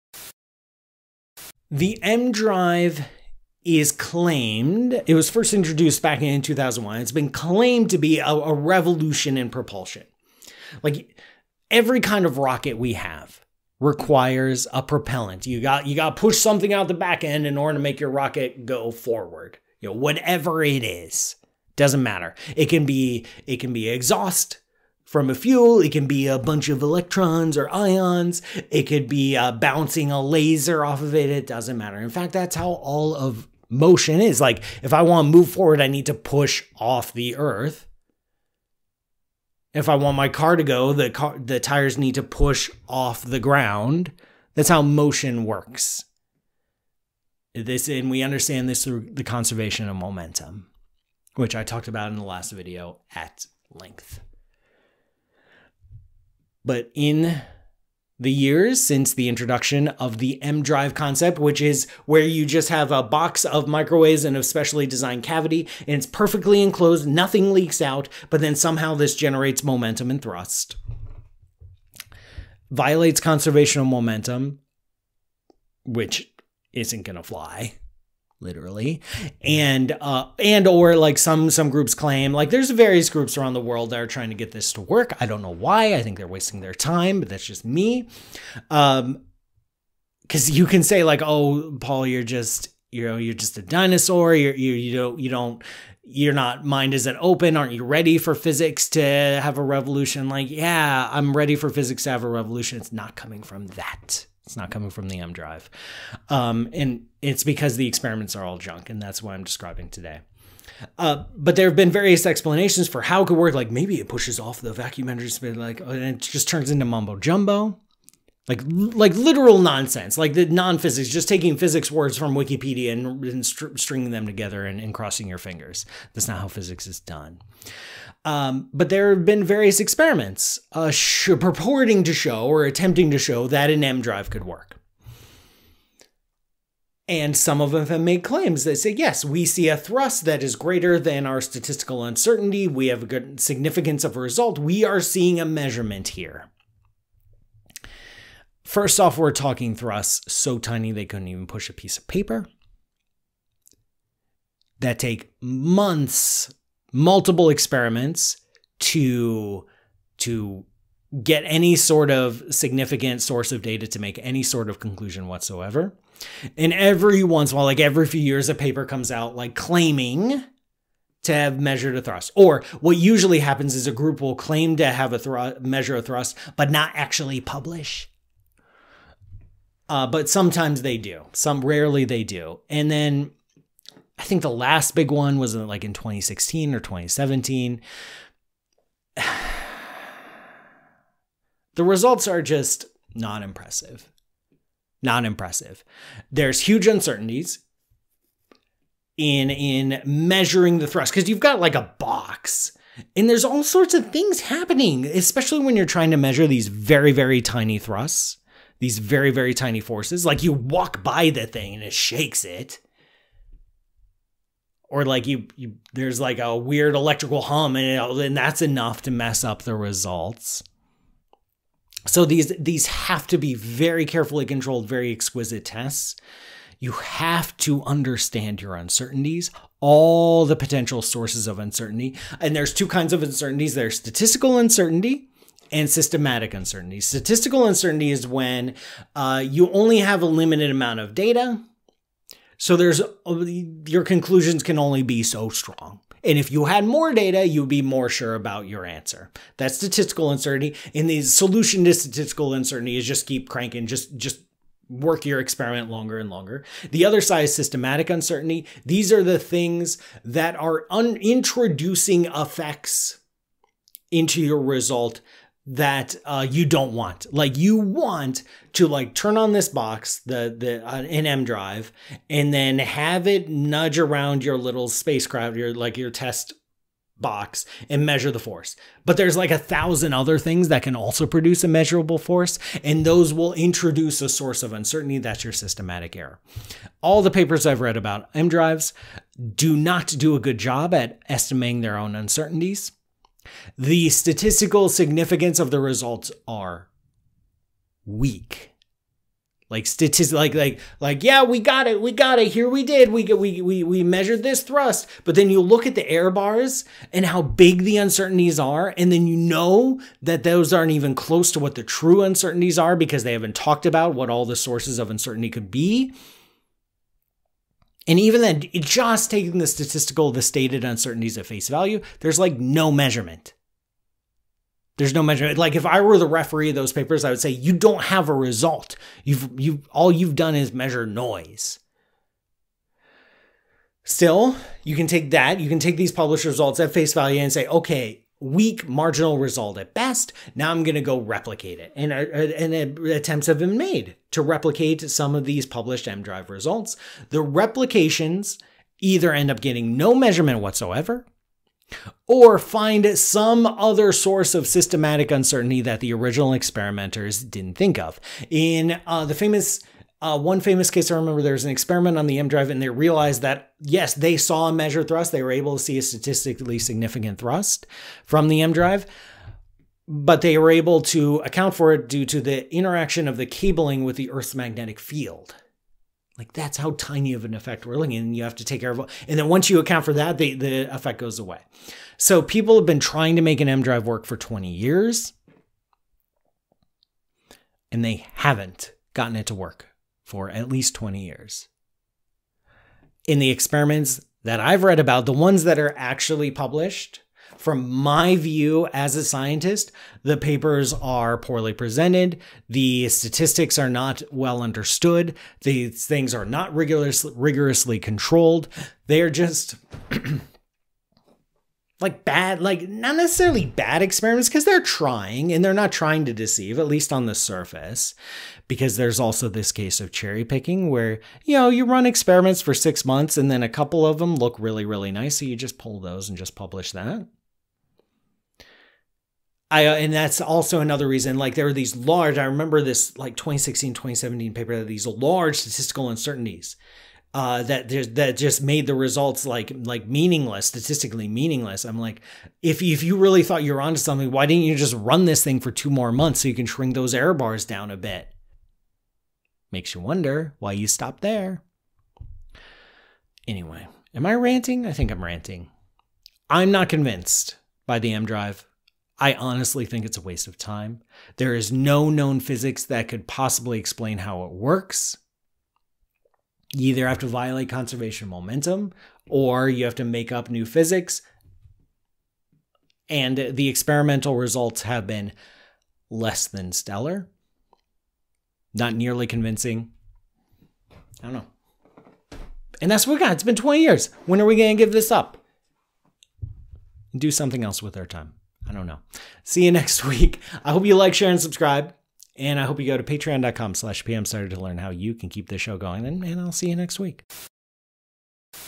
The EmDrive Experiments Were Junk - Ask a Spaceman!
What does the “emdrive” claim to do? How does it violate almost all of known physics? What were the problems with the experiments? I discuss these questions and more in today’s Ask a Spaceman!